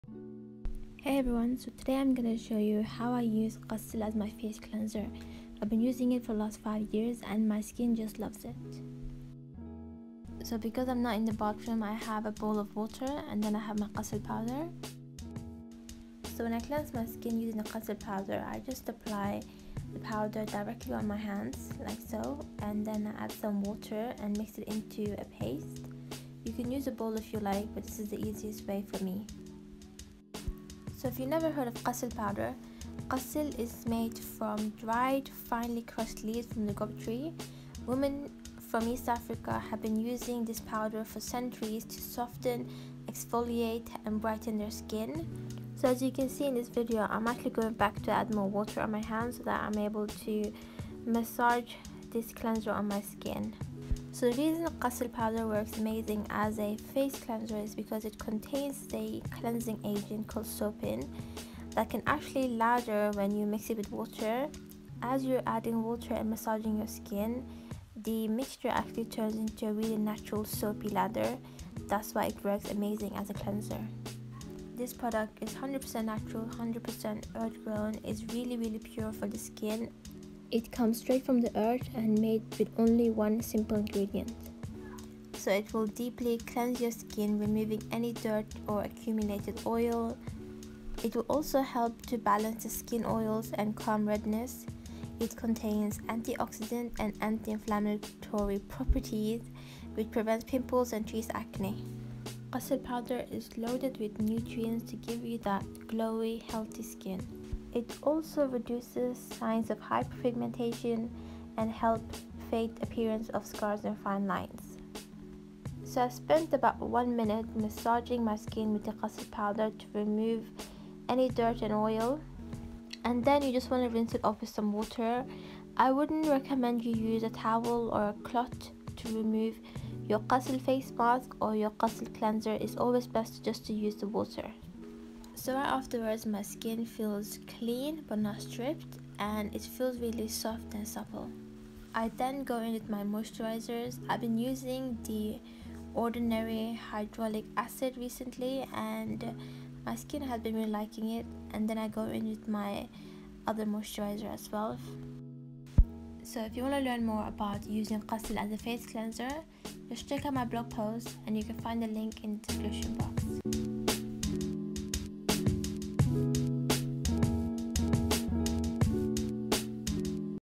HOW TO USE QASIL AS A FACE CLEANSER | Huda Organics
Qasil has been the only cleanser I have been using for the last 5 years. It really does a great job at removing dirt and oil, it also leaves my skin soft and smooth.